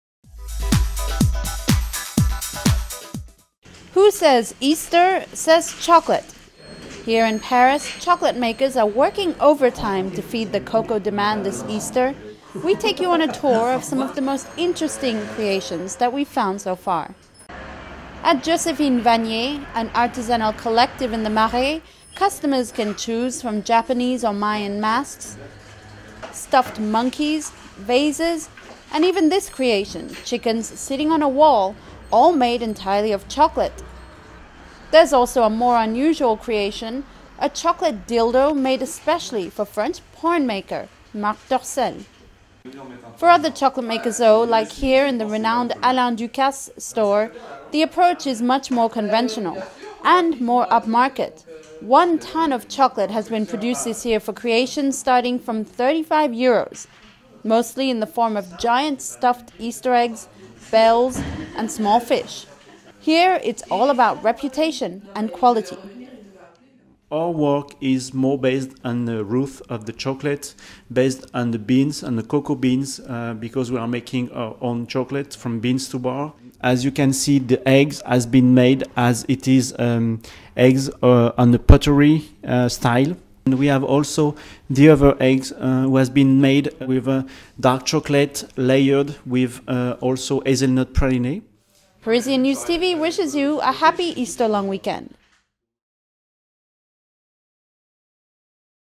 Chocolate dildos amongst Easter surprises in Paris
We took a tour of what's trending in Paris' chocolate shops this Easter...from the unconventional chocolate dildo for French pornmaker Marc Dorcel to the traditional Alain Ducasse, we discover what's on offer. By Parisian News TV.